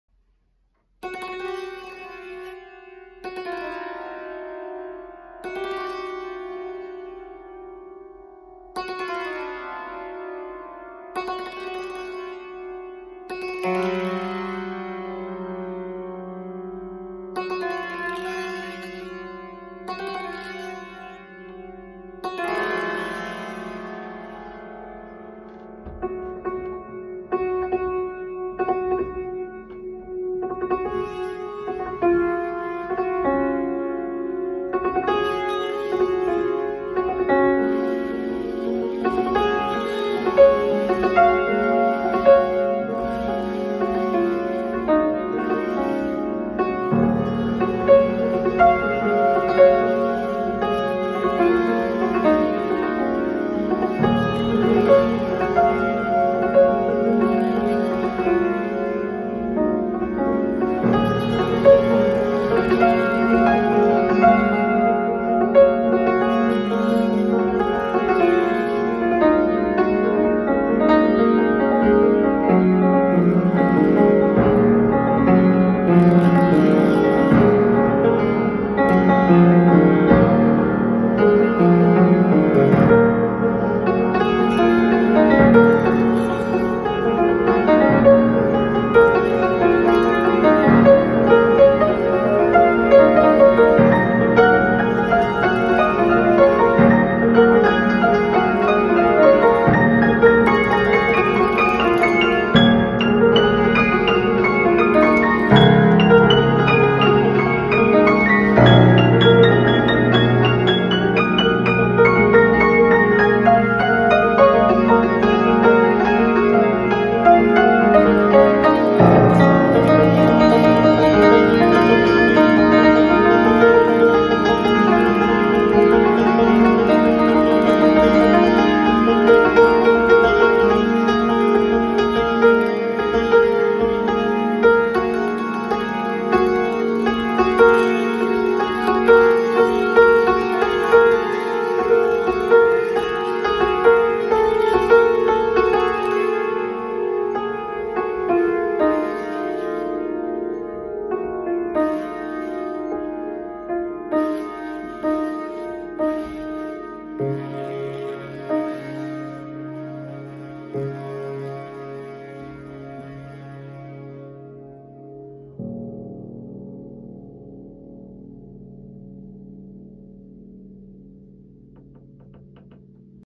London Lockdown with Ping Pong Balls & Piano
Inspired by the lockdown life in London where life is like a peaceful bubble at the chaotic time